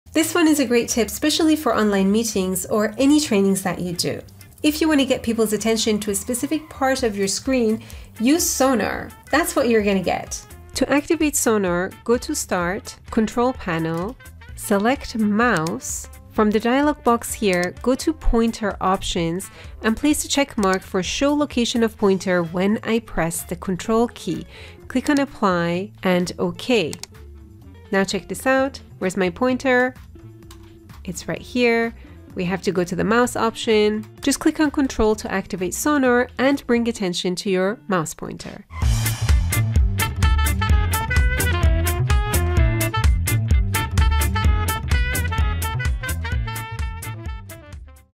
Presentation Tip: Use Mouse Sonar to Direct Attention
In this video you'll learn how to activate the "sonar" effect for your mouse. This way whenever you're presenting your screen (whether in-classroom training, online meeting, Excel training, etc.), you'll be able to direct the attention of your audience to where the mouse is. Just hold down the control key and click the left mouse button. Simple tip that gets your audience's attention and makes them wonder how you manage to do that!